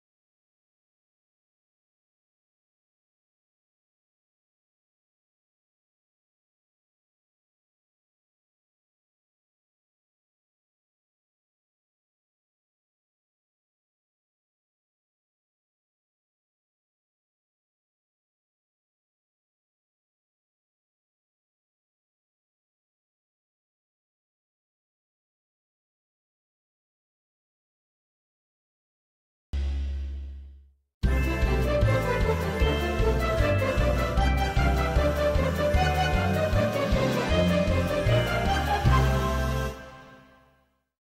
Extreme Yooperlite Hunting with the Convoy C8
If you are like me you only want the best when it comes to UV flashlights! Check out this quick video of our last extreme adventure hunting for Yooperlites at night using the Convoy C8 available